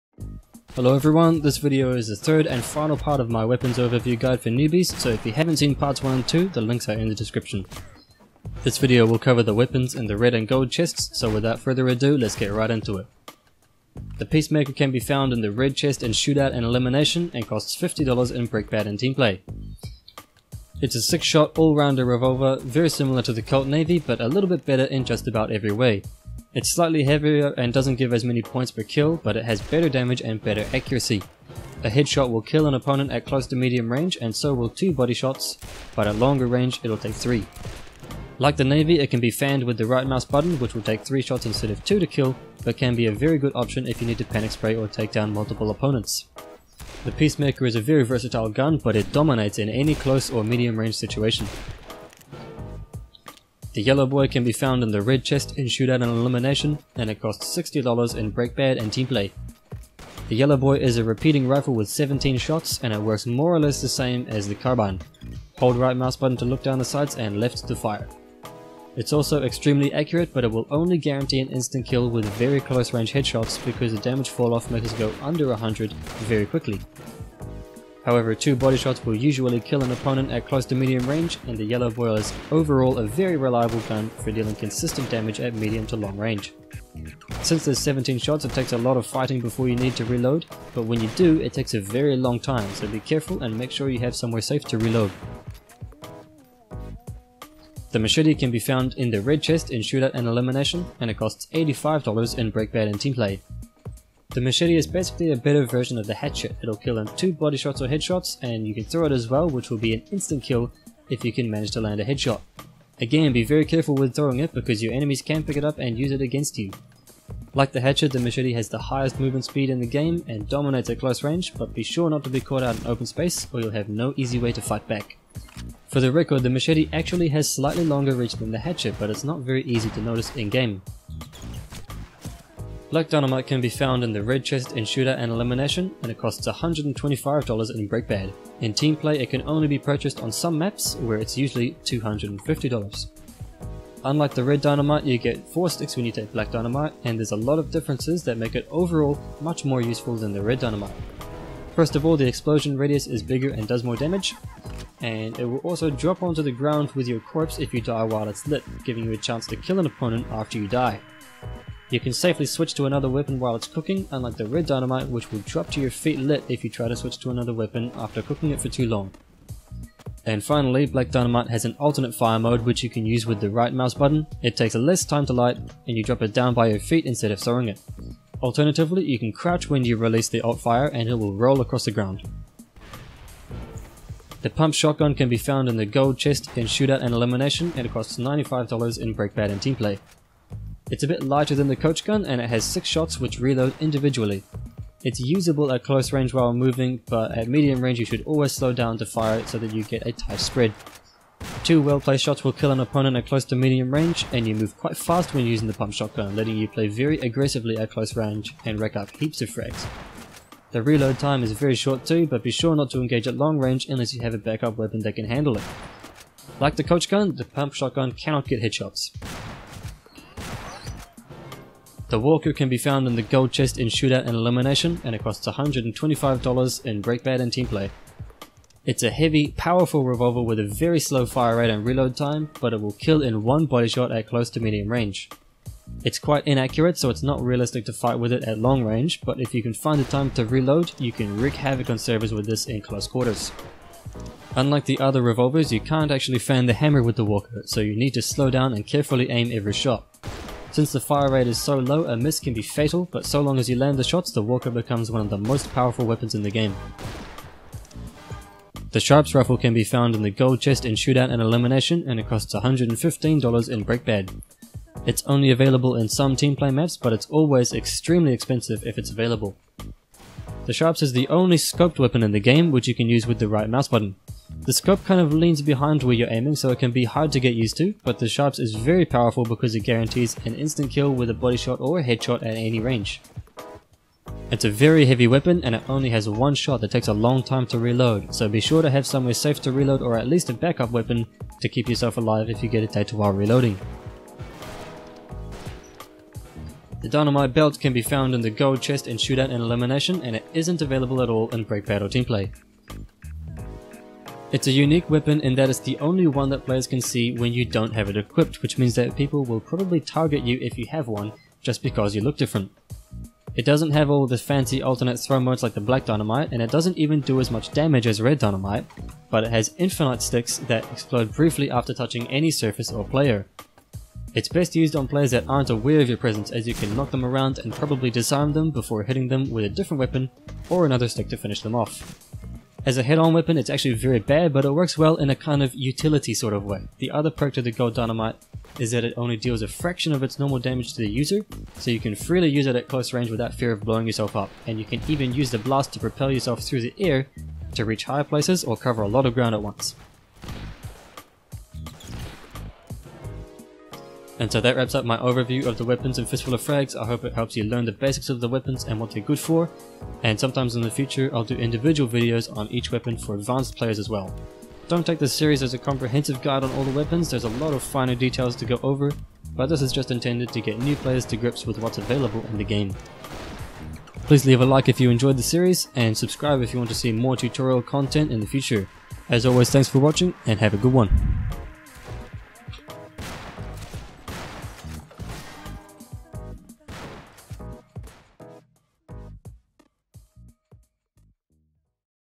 Newbie Guides - Weapons Overview, Part 3
Part 3 of 3 of my Weapons Overview videos, covering the basics of all the weapons in the game and how to use them. This episode covers the weapons from the red and gold chests.

These videos are intended for beginner players who are new to the game, hopefully it will help you get oriented with your arsenal and end a lot of confusion!